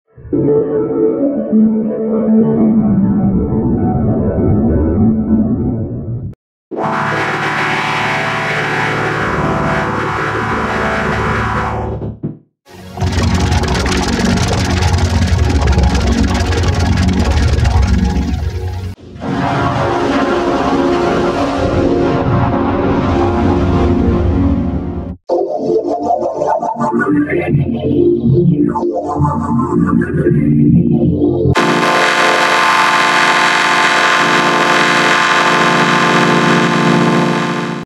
(2/2) (Kyoobur9000's 12th Anniversary Special) My 6 New Kyoobur9000's Series Effects
Happy 12th Anniversary To Kyoobur9000! (So I quit his company...)

(A.K.A. Stu Segall Productions - Film Strip (1992) Enhanced With My 6 New Kyoobur9000's Series Effects)

Effects List:
1. Free Spirit Glitch Chorded (sorry what I accidentally put Free Spirit Flanged Saw on that effect)
2. ChemicalFreeSpiritLights
3. 1.0 K-Points D U H
4. BluePower/K-PointsChorded Combo
5. 2554Jacquementup V1.Frosty
6. SwirlFrostyRays

How to make: Coming Soon...

and all the Fruit Lemon Lime Logos